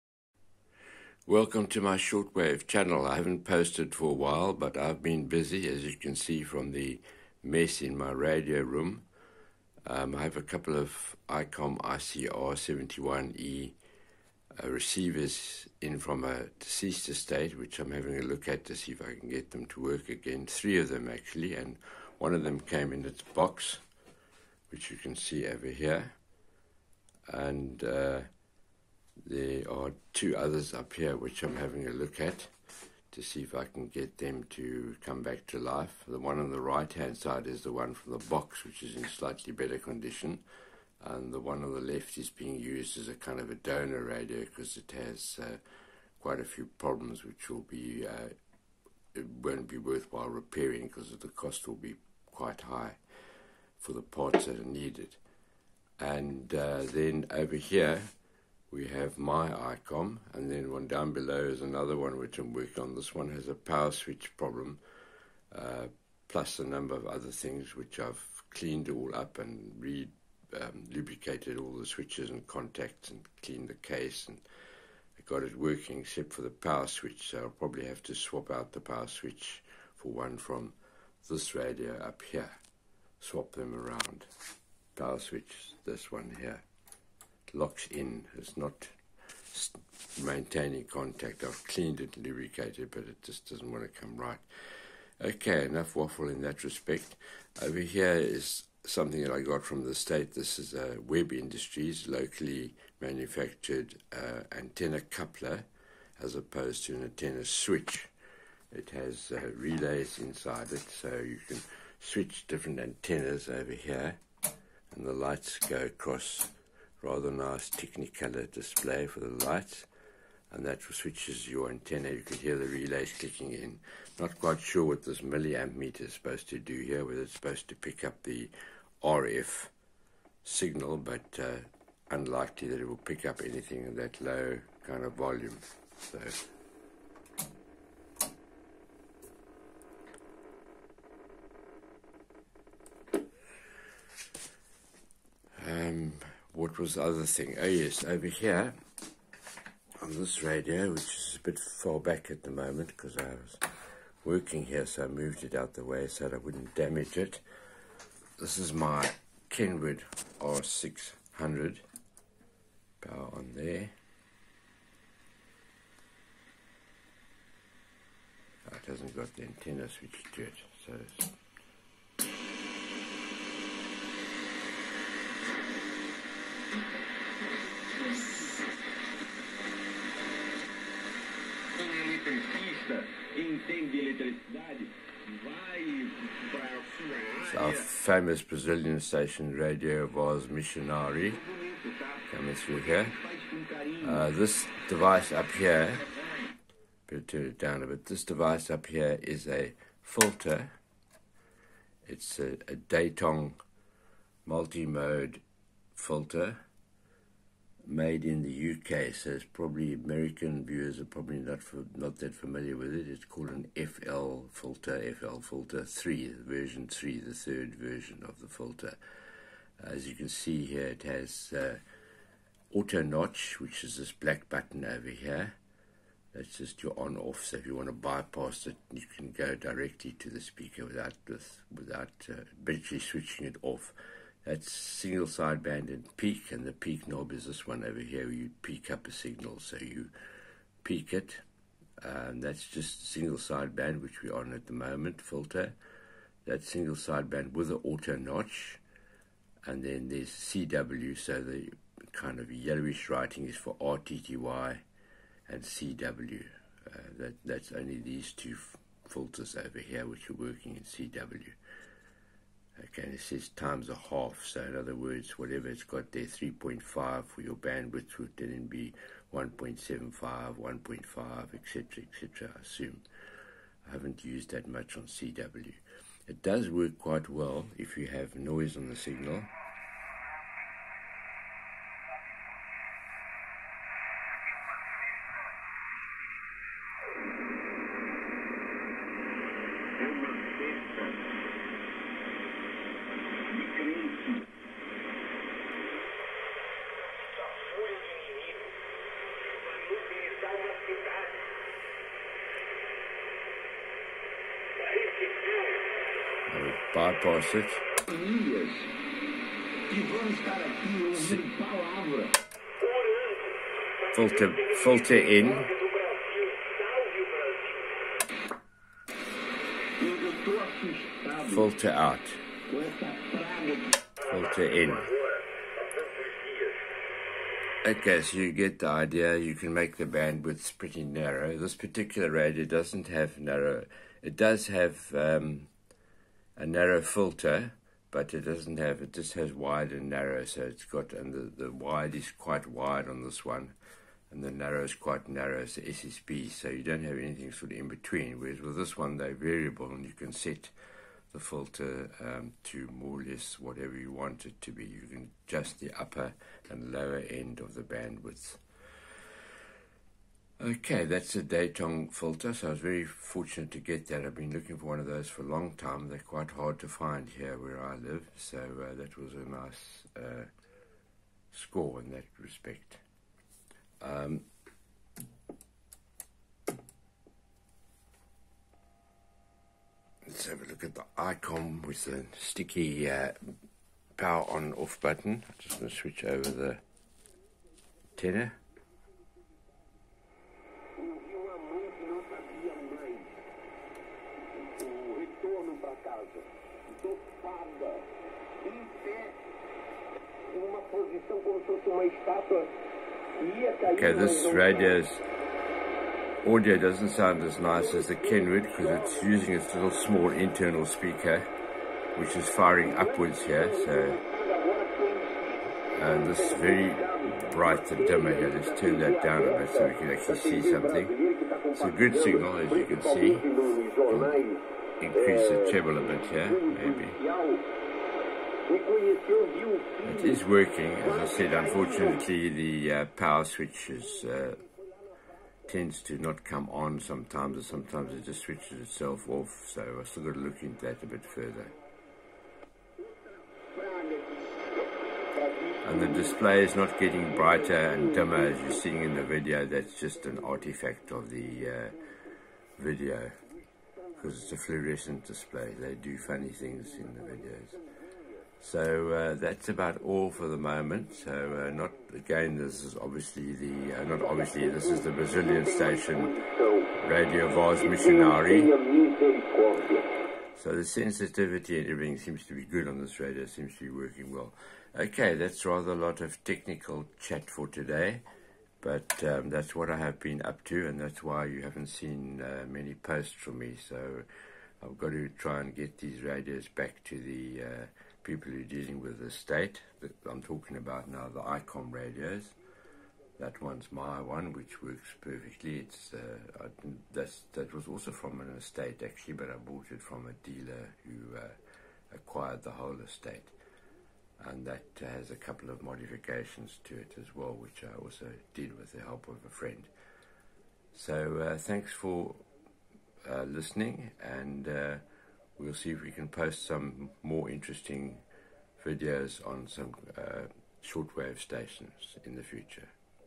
Icom IC-R71E overload and Datong filter
15/4/2023 at 0415 UTC

Icom IC-R72E Receivers in for repair
all three of them one with it's box.
Short demo of Datong FL3 filter
and Webb Industries antenna coupler which uses relays for the switching
one shortwave receiver to four antenna or one antenna to four radio receivers